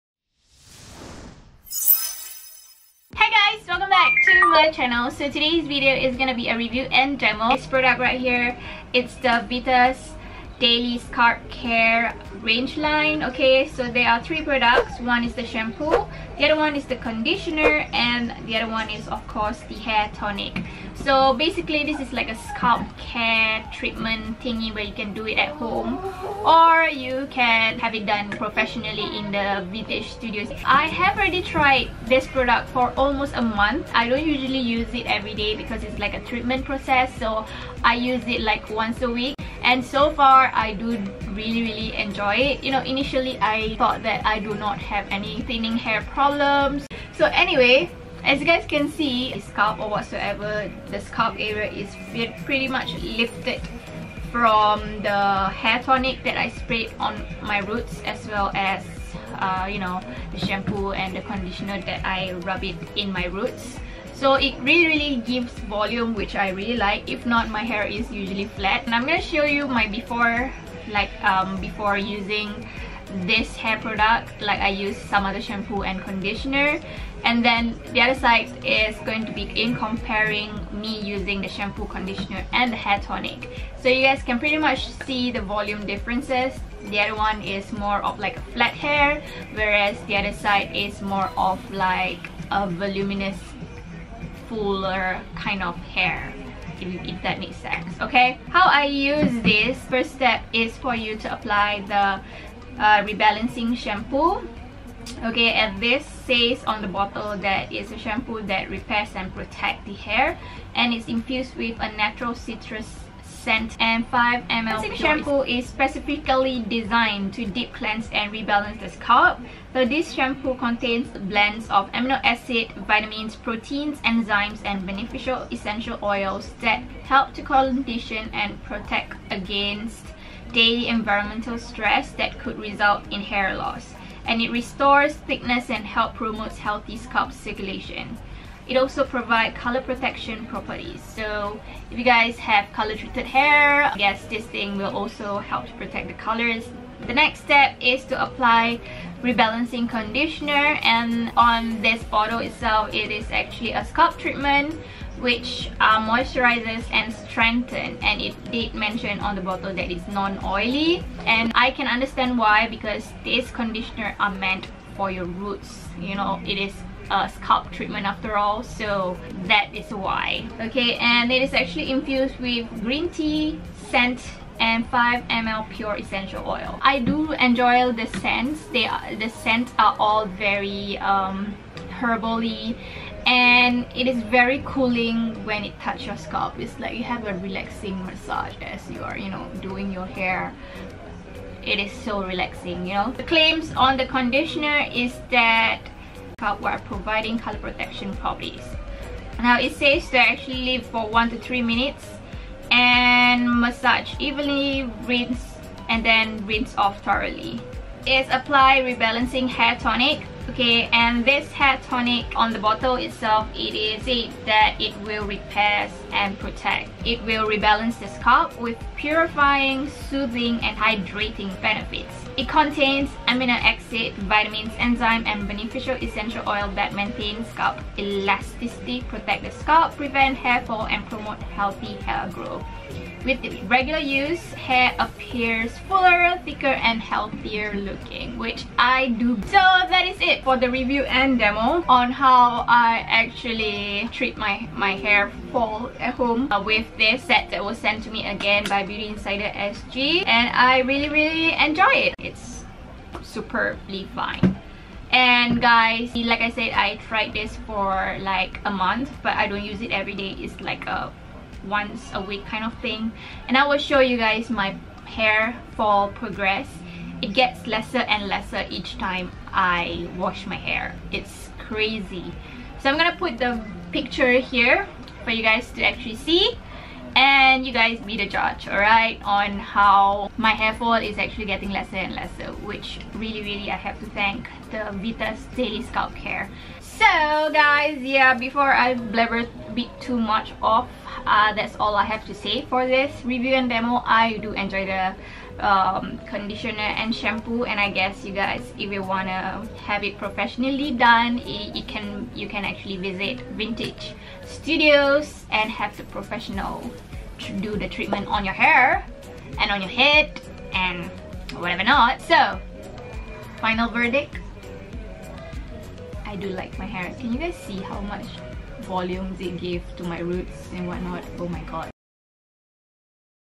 Tried, Tested & Loved : Singapore's Leading Organic Hair Product
Watch how our seeder deals with her scalp sensitivity and other issues found her thinning hair solution after just one month using VitasPro product. Real Testing, Real Reviews from the Beauty insider trial team.